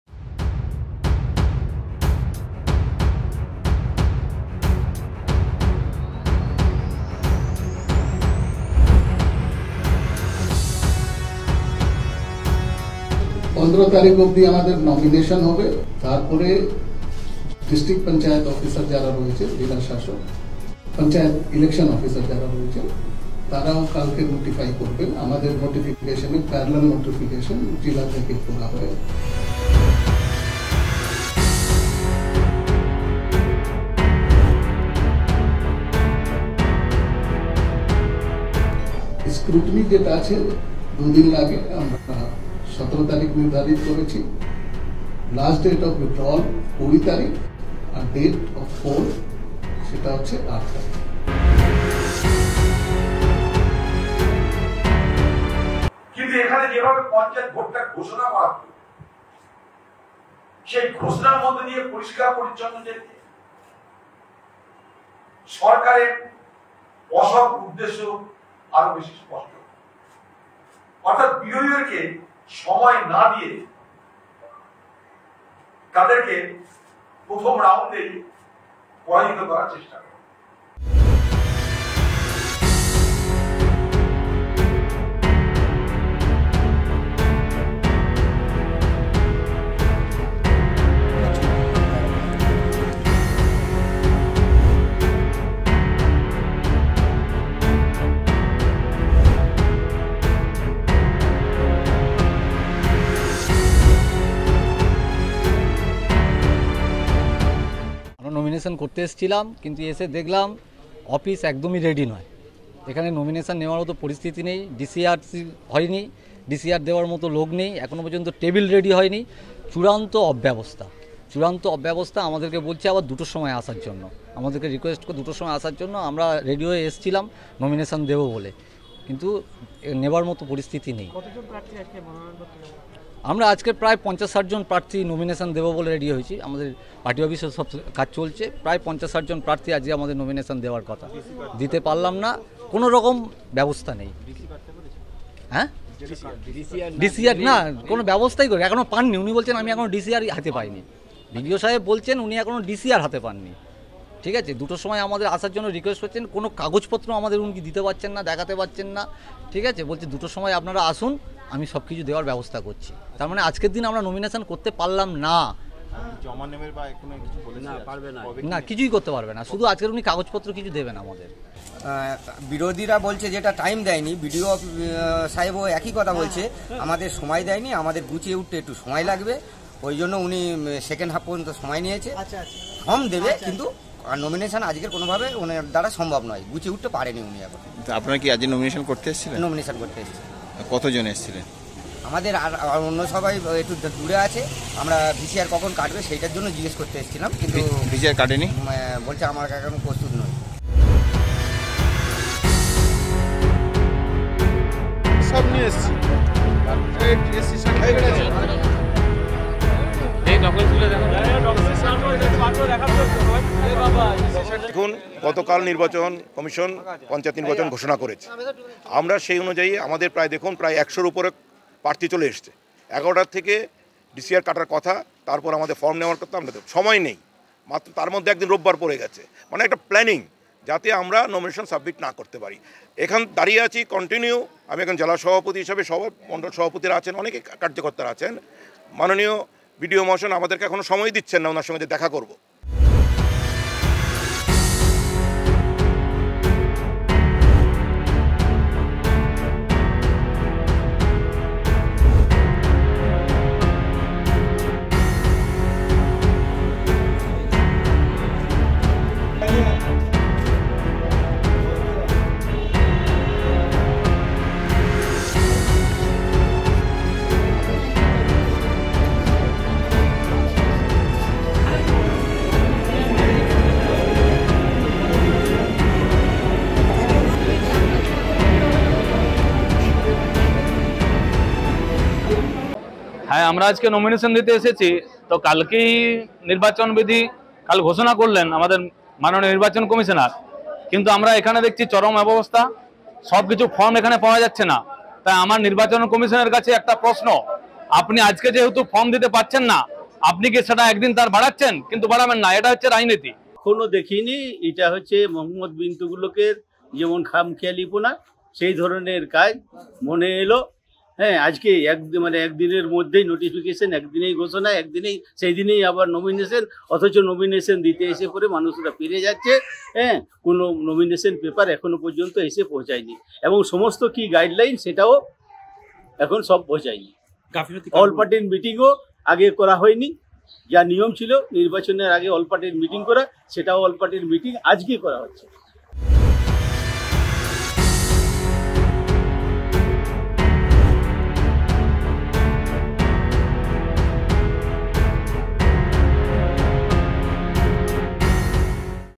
PanchayatElection2023: ‘মনোনয়ন নেওয়া যাবে না আজ’! সাফ জানাল প্রশাসন! | Ei Samay
ডায়মন্ড হারবারে মনোনয়ন জমা নিয়ে সমস্য়া। বেলা অবধি প্রয়োজনীয় কাগজপত্র পৌঁছয়নি। ব্লক প্রশাসন অসহায়, ক্ষুব্ধ বিরোধীরা।